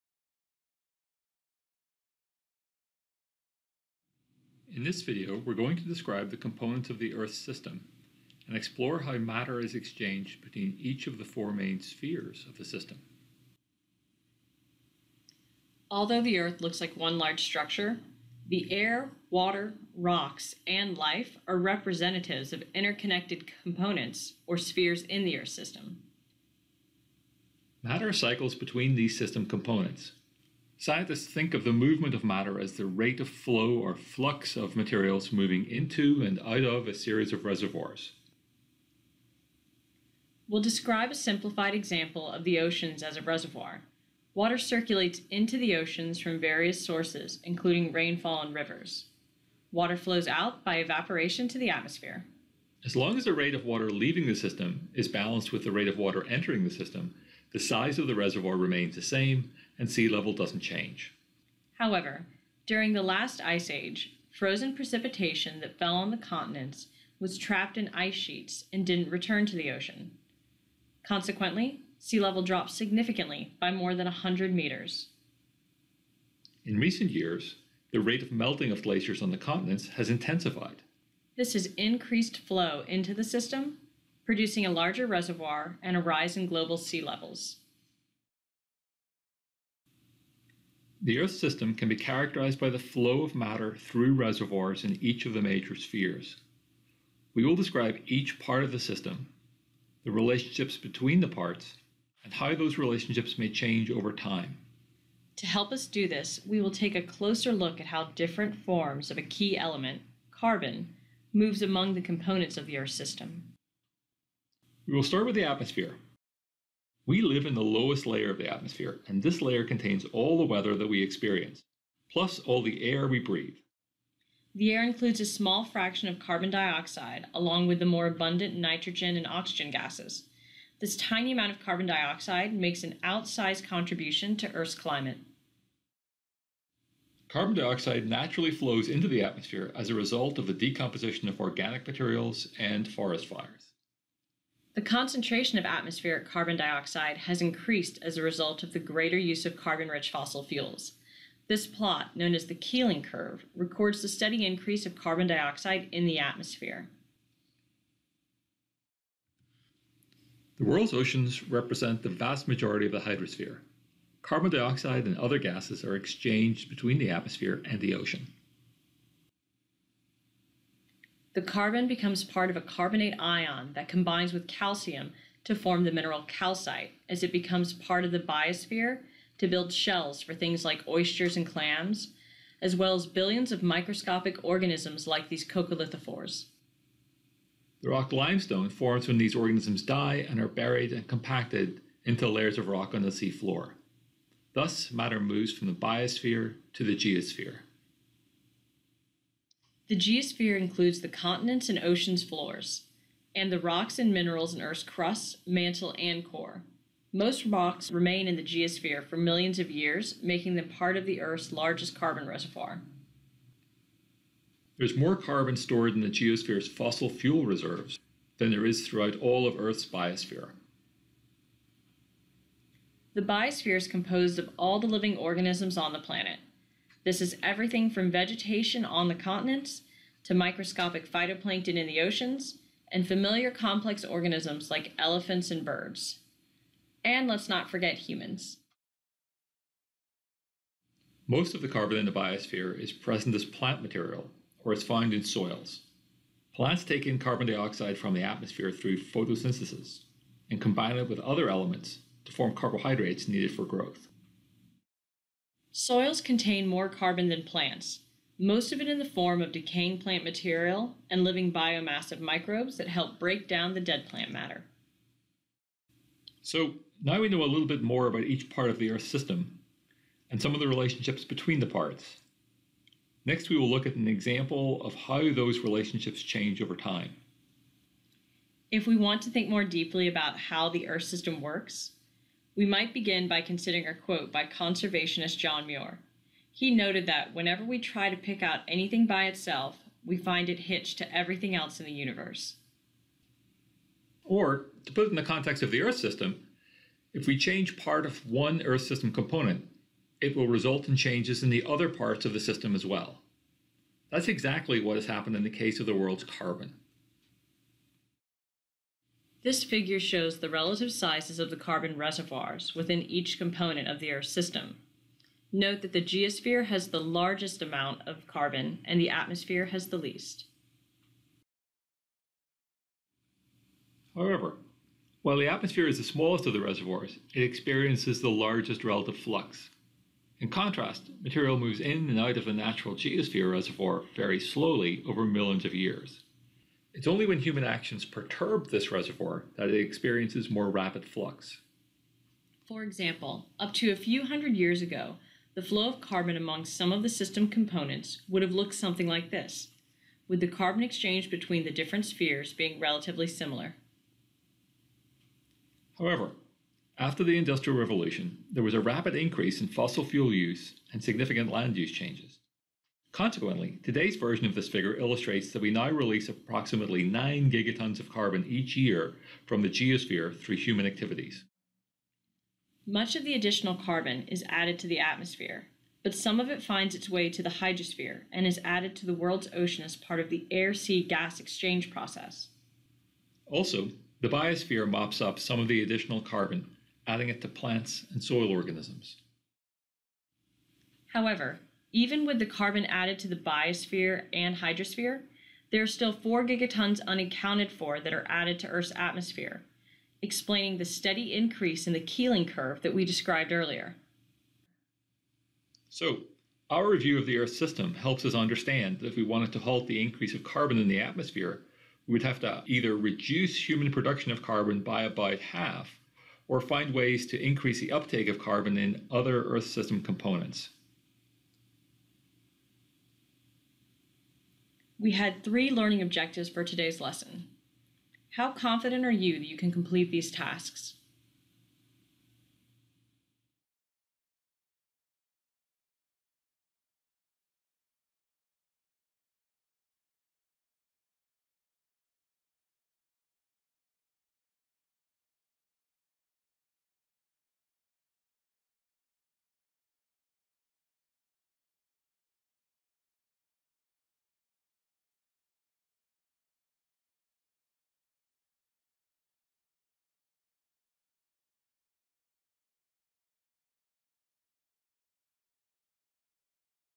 The Earth System
This video describes the four main components of the Earth system (atmosphere, biosphere, geosphere, hydrosphere), how matter is exchanged between the components, and how a change in some aspect of one part of the system will result in changes in other system components. We describe the Earth system in terms of reservoirs and flux between them and discuss how the movement of carbon occurs between system components.